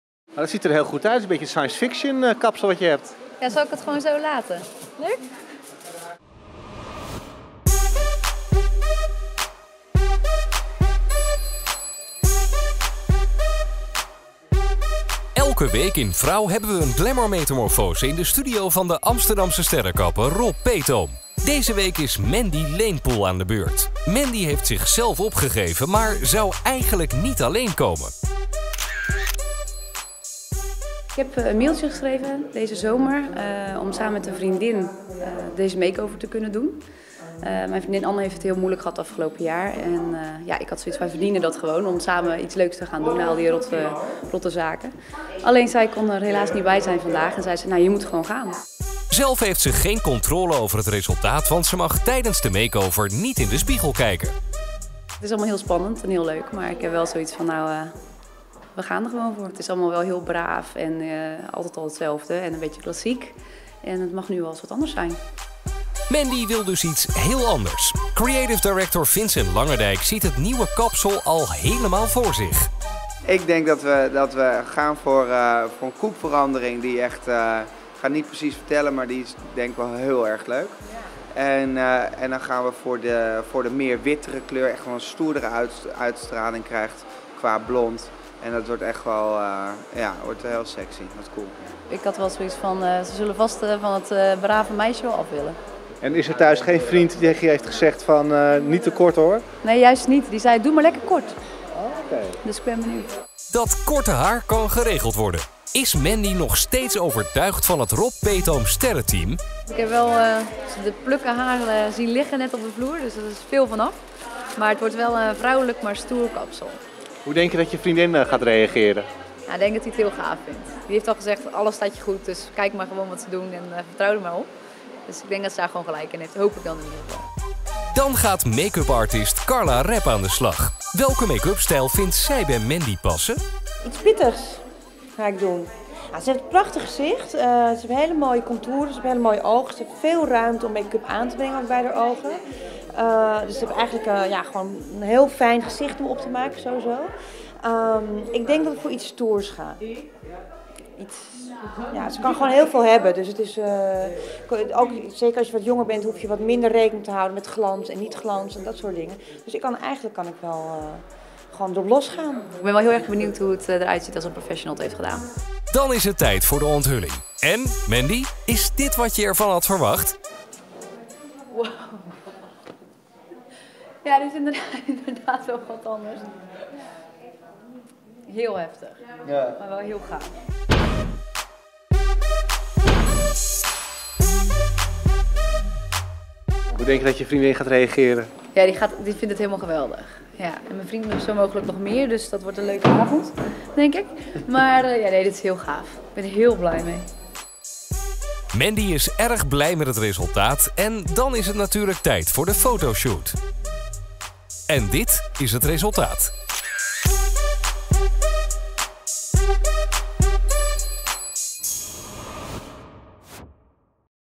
VROUW make-over Mandy Leenpoel
VIDEO Deze week is Mandy Leenpoel 'op haar mooist' in VROUW magazine. 'M'n vriend vindt dit geweldig, dus dat wordt een leuke avond!'